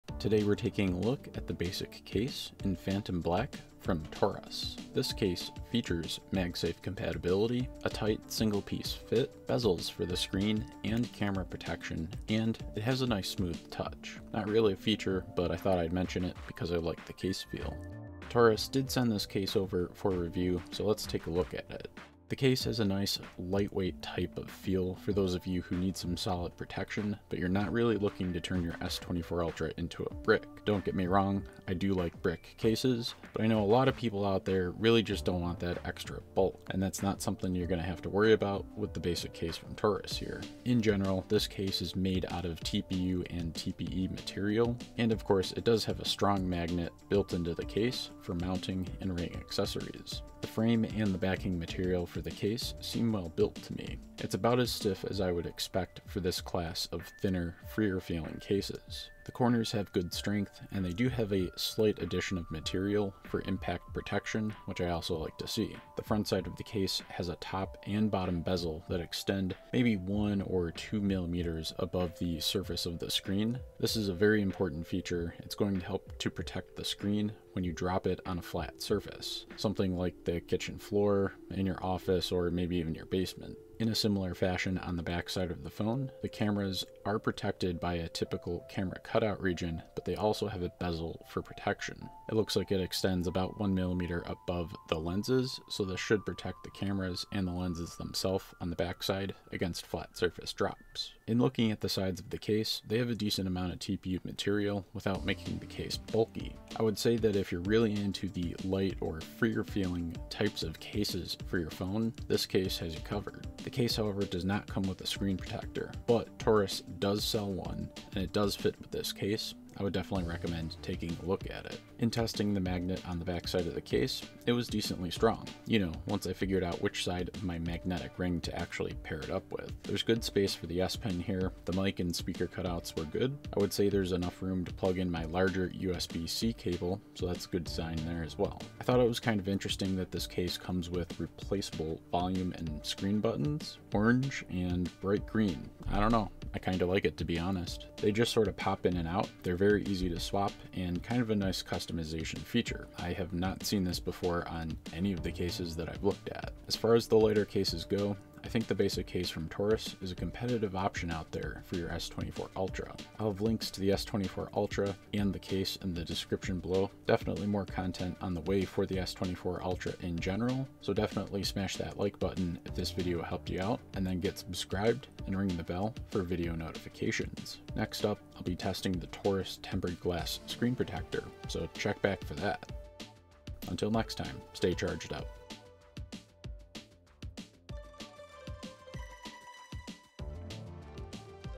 Torras S24 Ultra Basic case review
Torras S24 ultra Basic Case
Samsung Galaxy S24 Ultra

Today we’re taking a look at the Basic case in Phantom Black from Torras. This case features magsafe compatibility, a tight single piece fit, bezels for screen and camera protection, and it has a nice smooth touch, not a feature but I liked the case feel. The basic case is a nice light weight type of case for those of you who need some solid protection but aren't looking to turn your S24 Ultra into a brick. The case is generally made of TPU and TPE material and of course has the Neodymium ring magnet built into the case for mounting and ring accessories. The frame and backing of the case seem well built, about as stiff as I would expect for this class of thinner or freer feeling cases. The corners have good strength and a slight addition of material for impact protection. The frontside of the case has a top and bottom bezel that extend maybe one or 2mm above the surface of the screen. This will help prevent the screen from making contact when you drop your phone on a flat surface. Similarly, the cameras have the typical camera cut out region with a bezel for protection. This should keep the cameras and lenses safe when you drop your phone on the floor for example. The sides of the case have a decent amount of TPU material without being bulky so yea, I would say if you like that light free feeling for your phones, this case has you covered. It doesn't come with a screen protector but Torras does sell some nice screen protector kits that I would definitely pair up with this case. The magnet on the backside was decently strong once I figured out which side of my magnetic ring to pair it up with. There’s good space for the S-pen here, the mic and speaker cutouts are good, and there's enough room to plug in my larger USB-c cable so good design work there. I thought it was kind of interesting that this case comes with replaceable buttons? Orange and bright green, I kinda like it to be honest. They just sort of pop in and out, very easy to swap. As far as lighter cases go, I think the Basic from Torras is a competitive option for your S24 Ultra. I will be testing the Torras tempered glass screen protector soon so check back for that. Until next time, stay charged up.